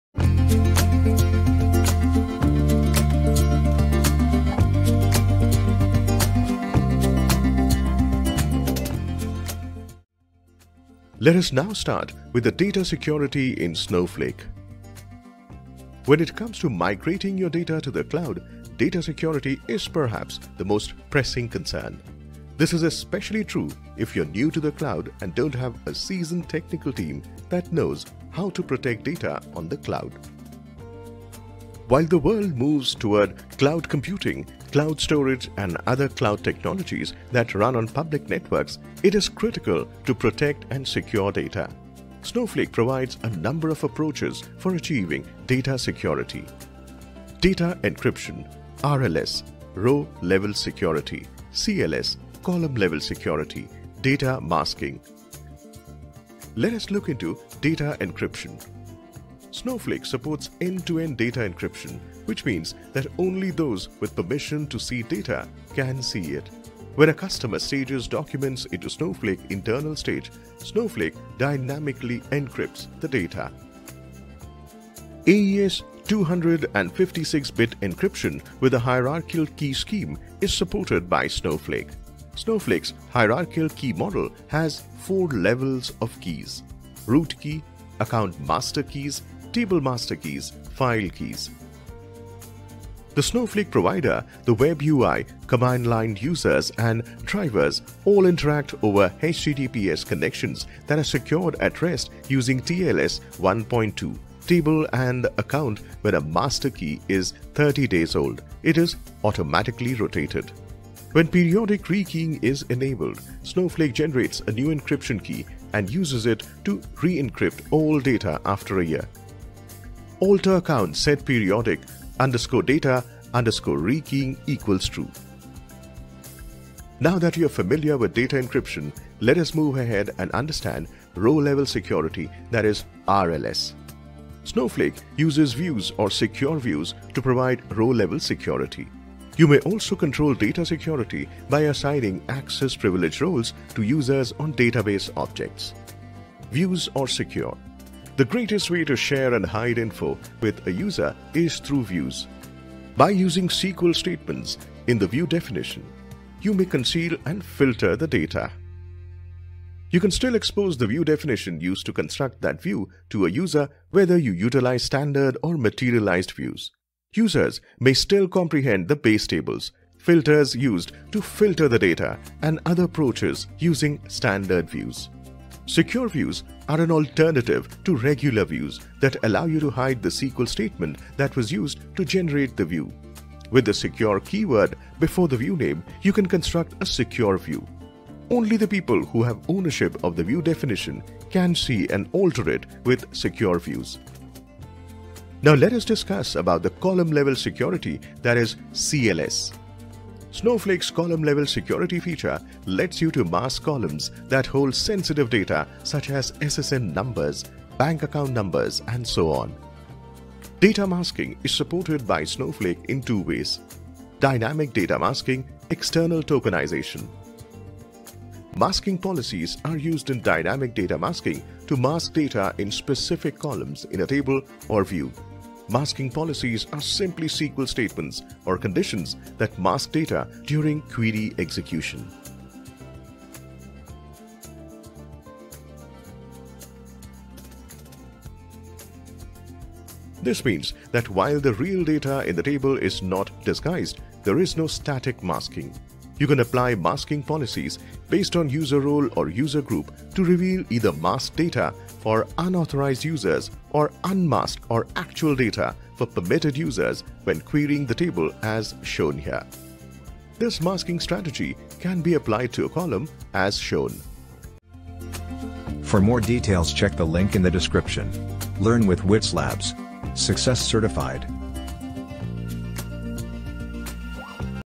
What is Snowflake Data Security: Learn Data Encryption, Row level Security, Column level Security
In this video, you'll learn all about Snowflake's advanced data security features, including encryption, row-level security, and column-level security. Discover how Snowflake enables you to protect your sensitive information and ensure compliance with regulations, all while delivering high performance and scalability.